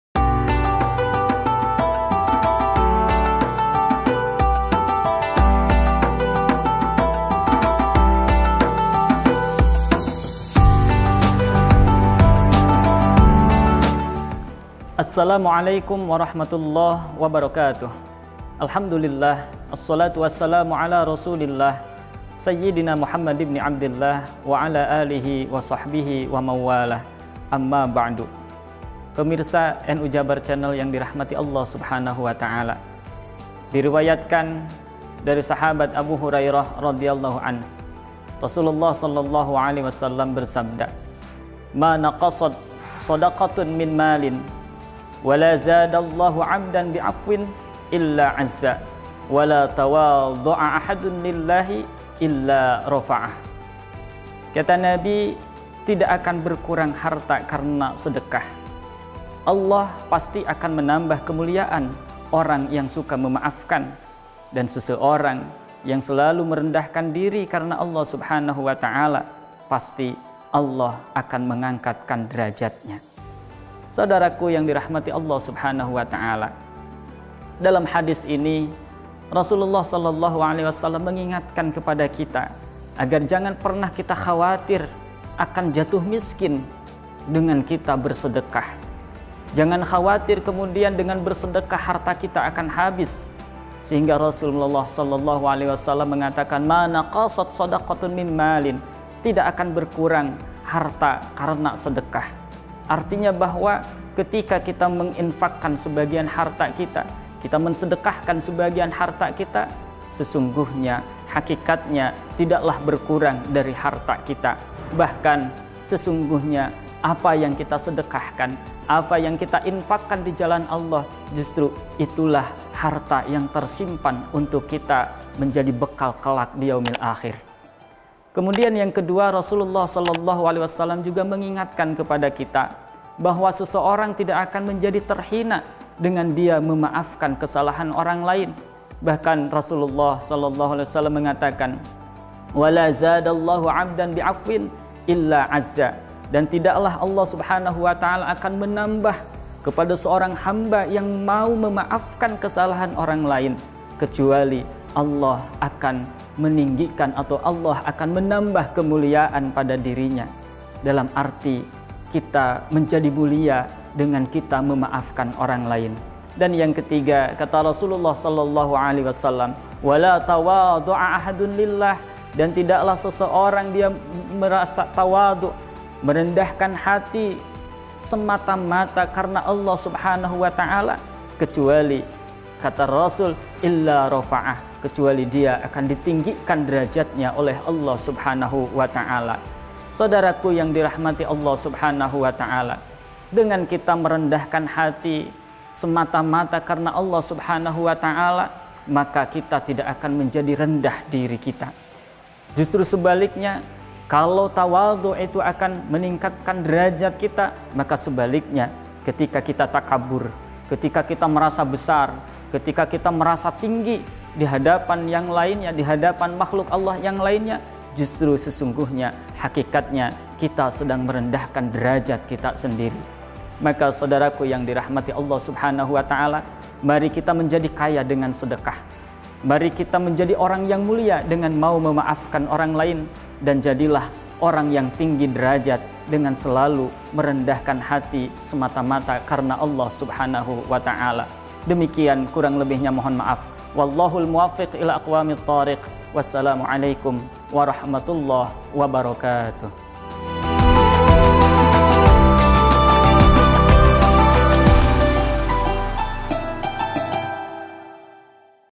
BERKAH RAMADHAN | NGAOS SORE #16 BERSAMA AJENGAN ENDI SUHENDI | MULYA DENGAN MEMAAFKAN
Program BerkahRamadhan​ Tahun 2021 M / 1442 H
Ngaos Sore bersama Kang Ajengan Endi Suhendi, M.Pd. (Sekretaris RMI PWNU Jawa Barat)